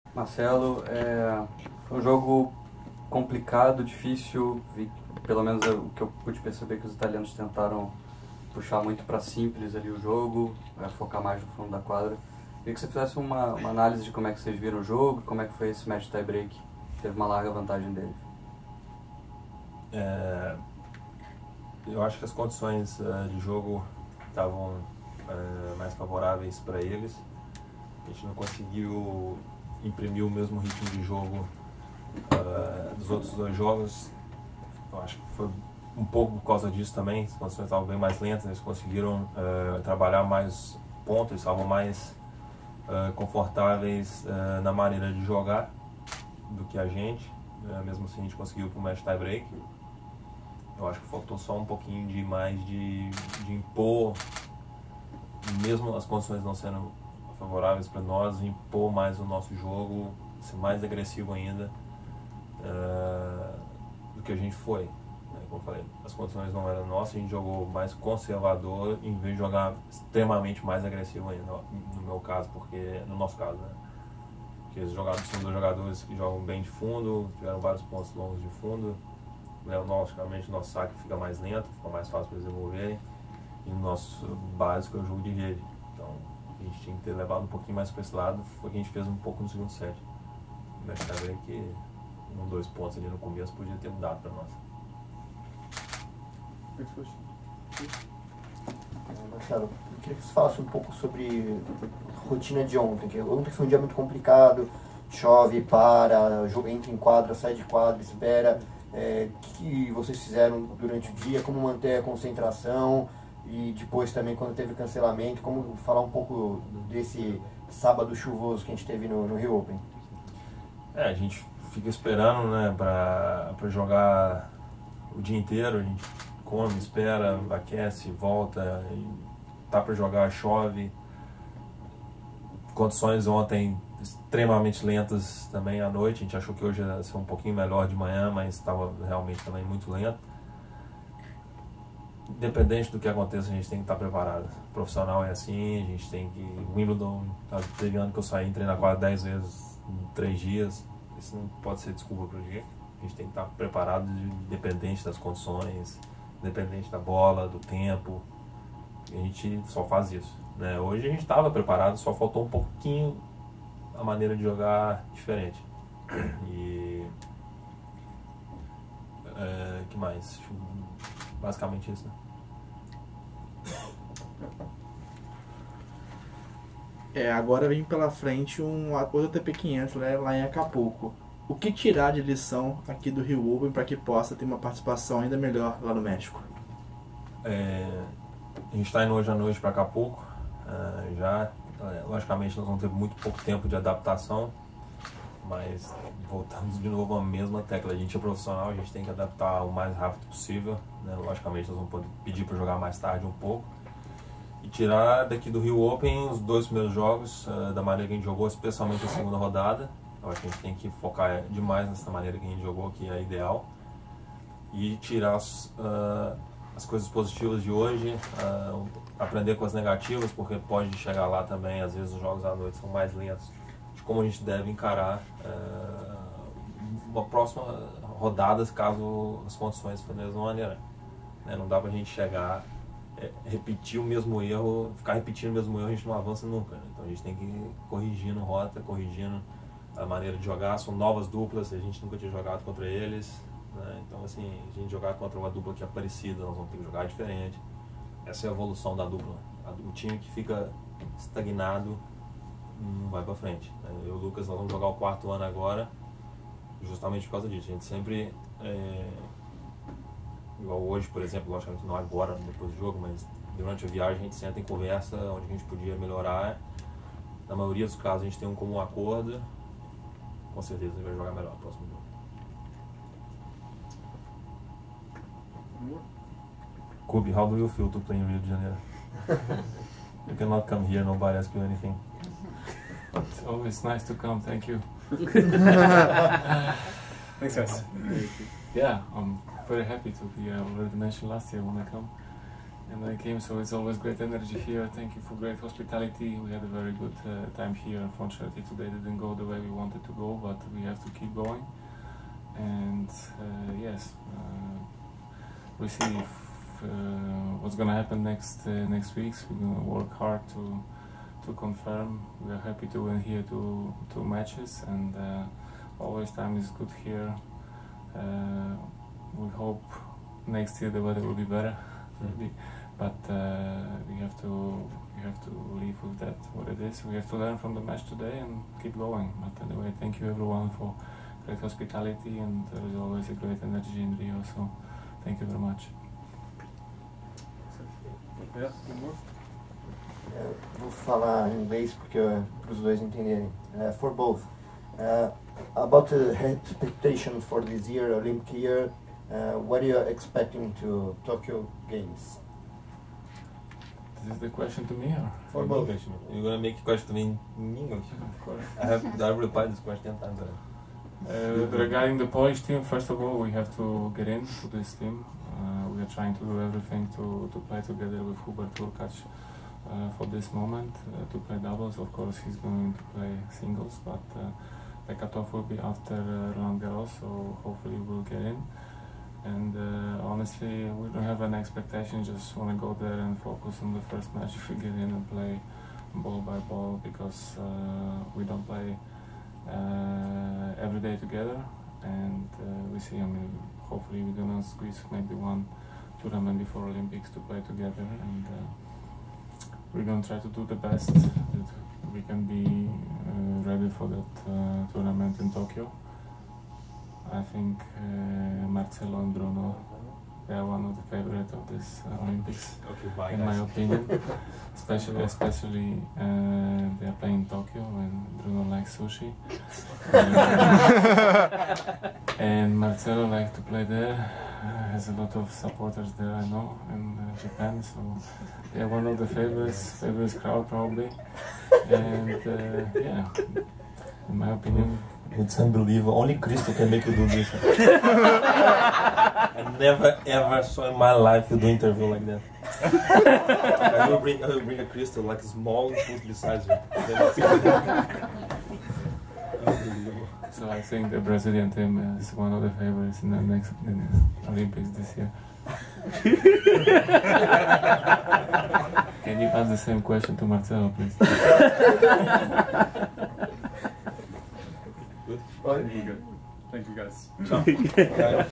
Coletiva de Imprensa de Marcelo Melo e Lukazs Kubot - Rio Open 2020 - 23/02
Confira a entrevista coletiva da dupla formada pelo brasileiro Marcelo Melo e o polonês Lukazs Kubot após serem derrotados na semifinal do Rio Open 2020 para os italianos Salvatore Caruso e Federico Gaio.

A dupla jogará agora o ATP 500 de Acapulco, no México.
Eles também comentaram o fato de ser um ano de Olimpíadas.